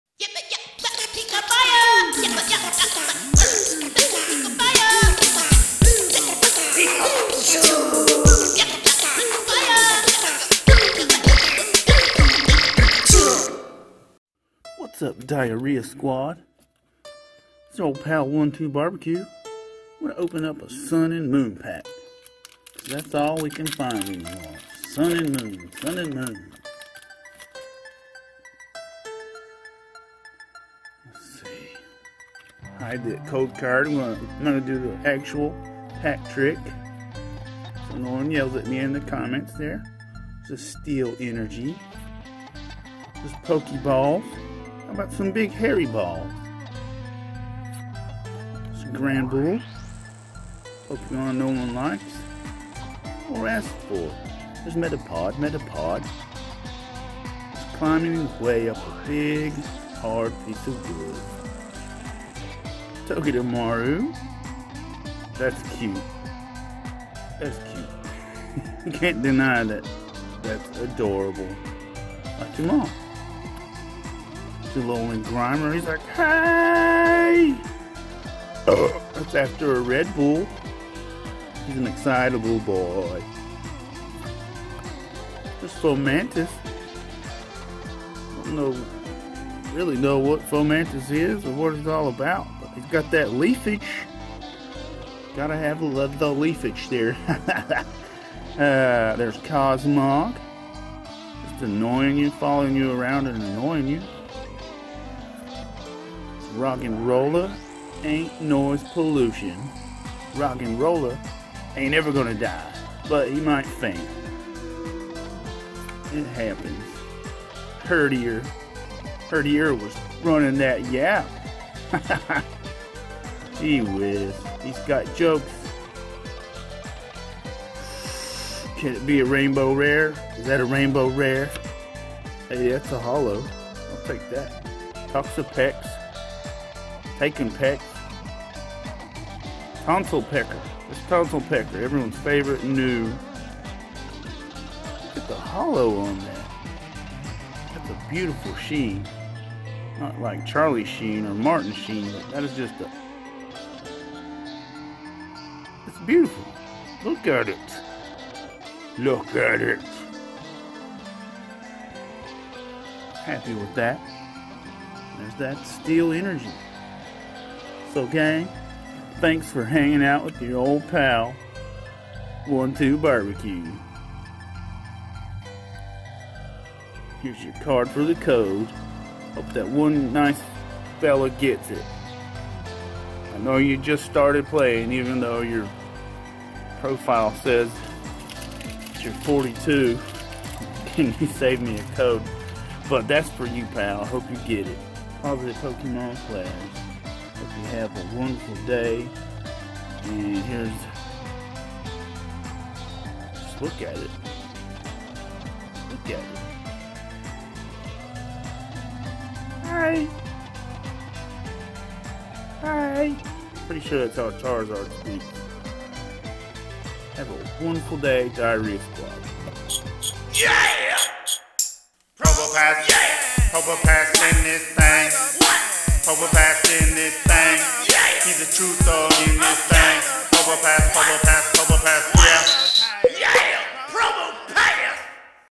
POKEMON SUN and MOON base set Packy!
Let's open a packy and hang out for awhile!
Free code for one lucky viewer!